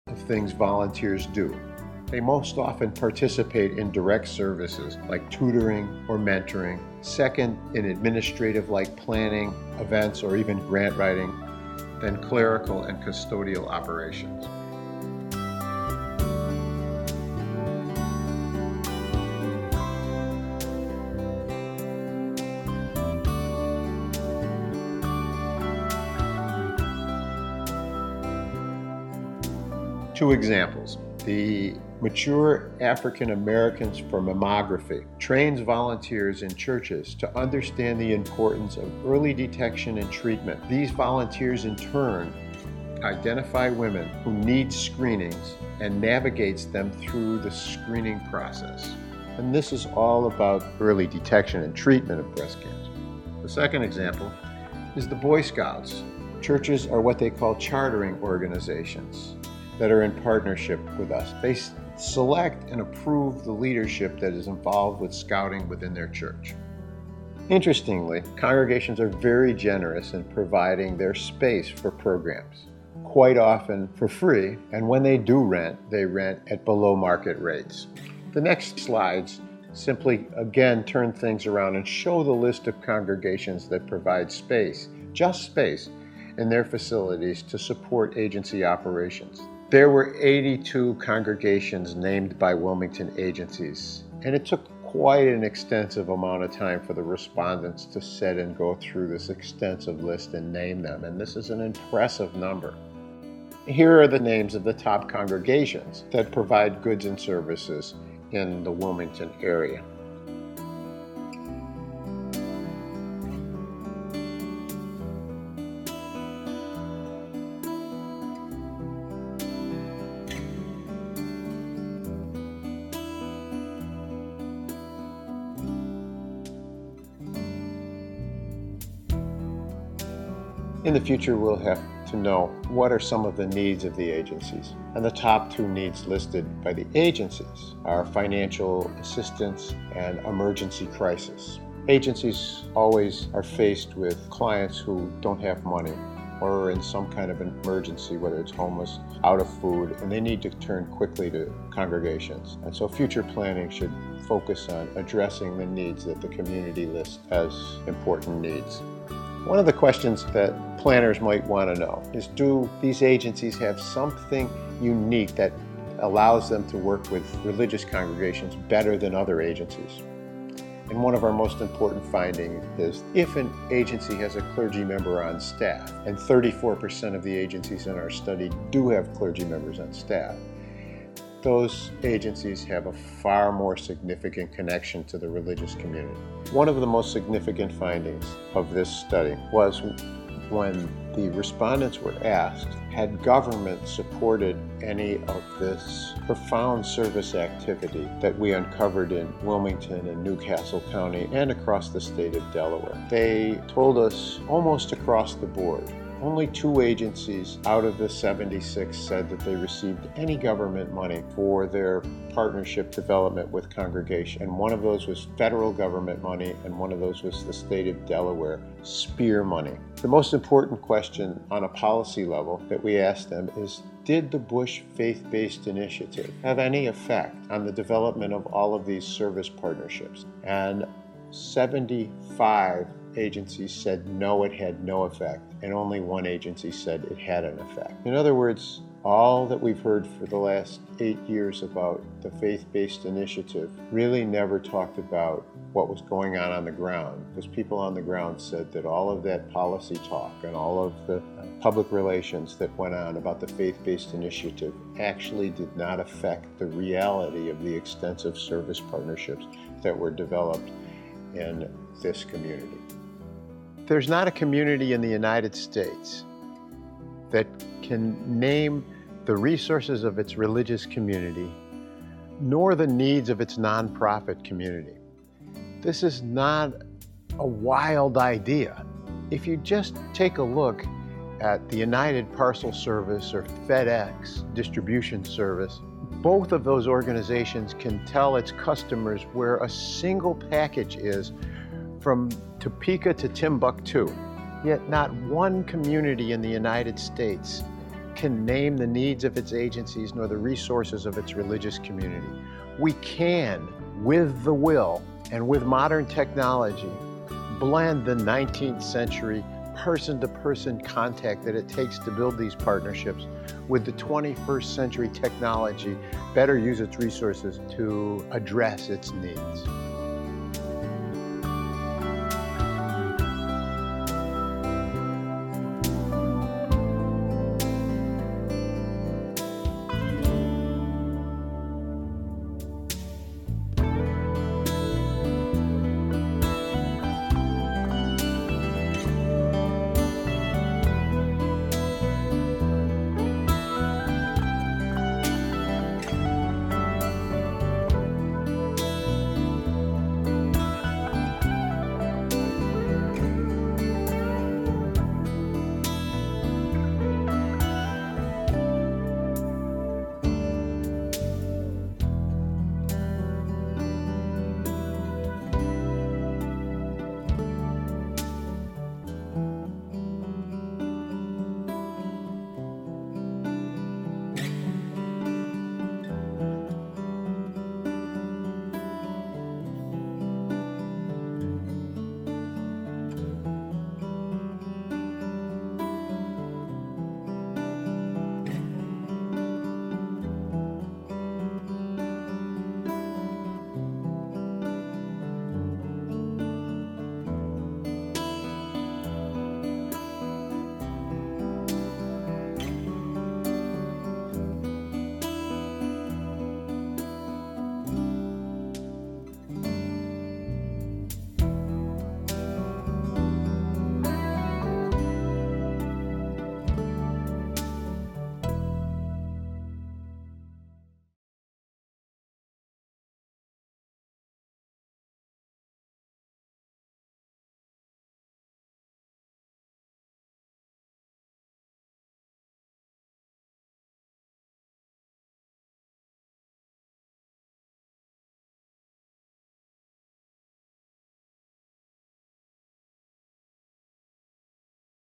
Faith Based Initiative/Social Service Research Part 2
Study of partnerships between social service agencies and religious congregations in Wilmington Delaware